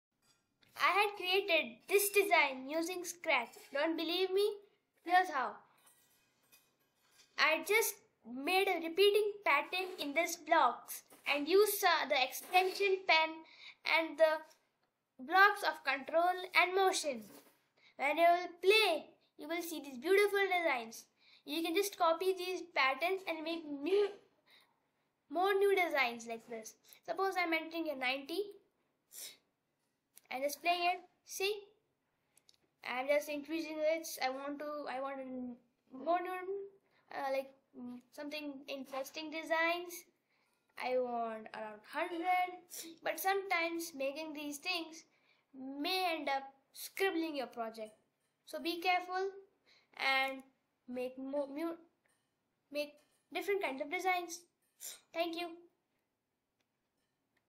Create Eye catching designs in scratch.
LOVE drawing❤❤?
So, this video is for you to start making unique designs using scratch on your computer screen don't worry it's easy, In my next video I will tell you how to draw using scratch, so do subscribe to my channel.